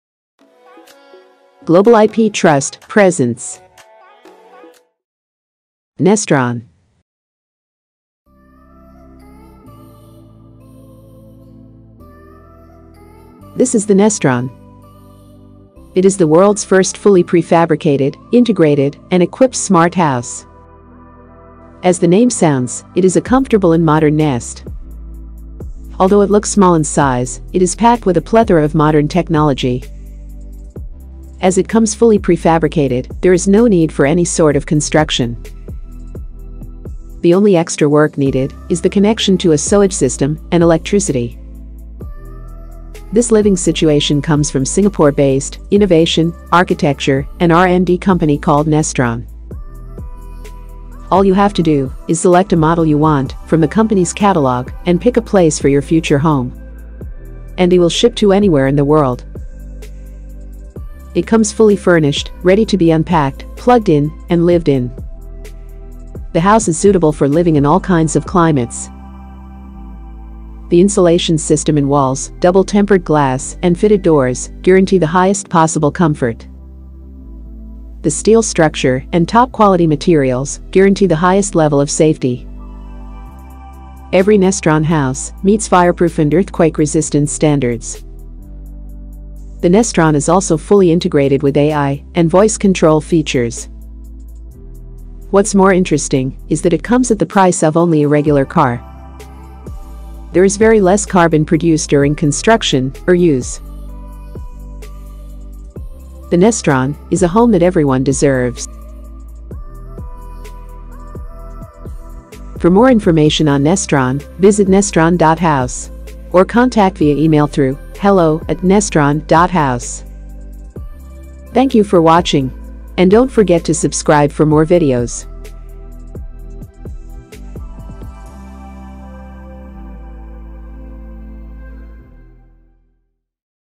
World's First Fully Integrated Prefabricated Smart House
The Nestron is the world's first fully integrated and prefabricated smart house. This smart house may be small, but it houses state-of-the-art technology and amenities making it extremely comfortable. All you need is a sewage and electricity connection and you are ready to live anywhere in the world.